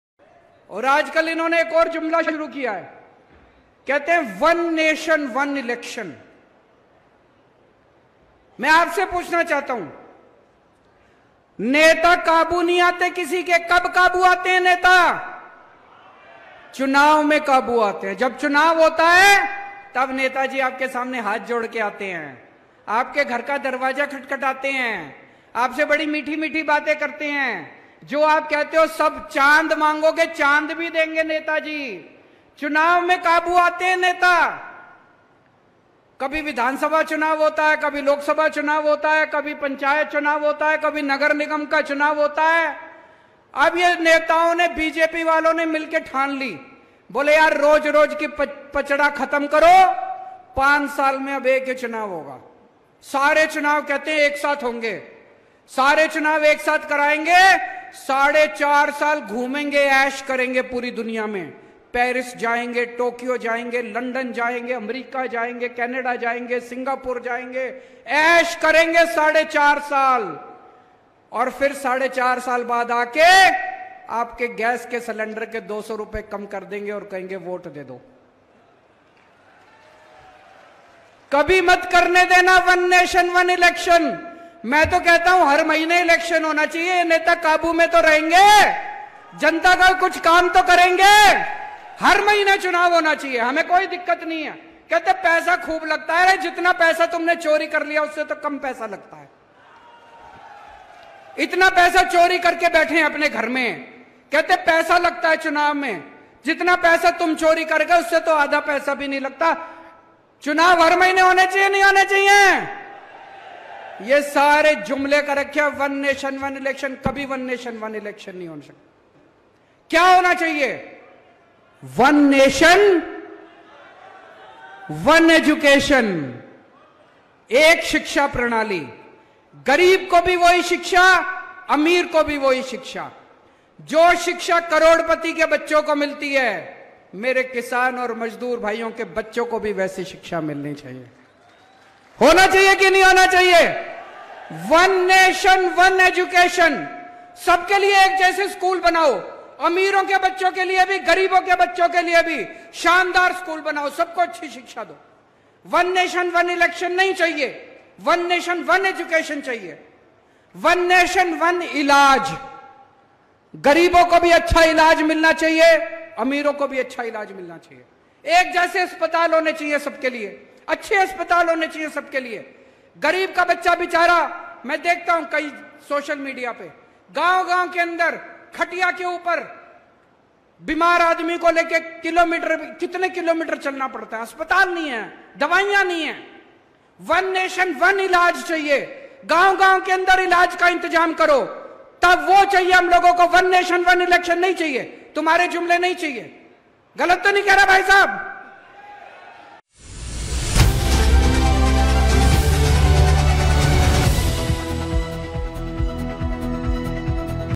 One Nation One Election नहीं One Nation One Education चाहिए - केजरीवाल | Haryana Election | 2024
एक देश एक इलेक्शन नहीं एक राष्ट्र एक शिक्षा चाहिए - Kejriwal | Haryana Election | 2024